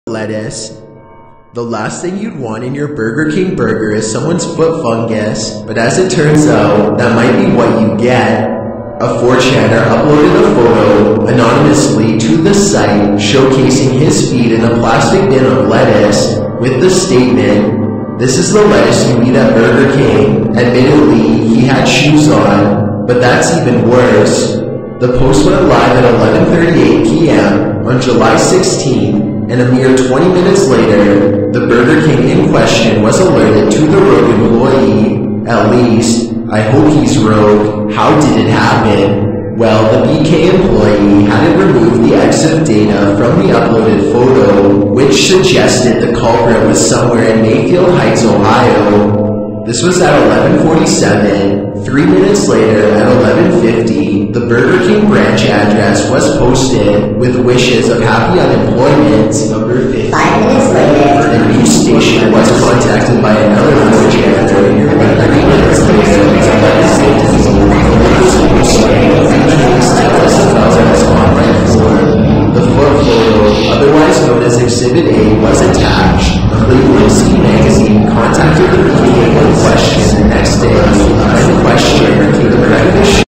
number 15: burger king foot lettuce
the last thing you want in your burger king burger is someones foot fungis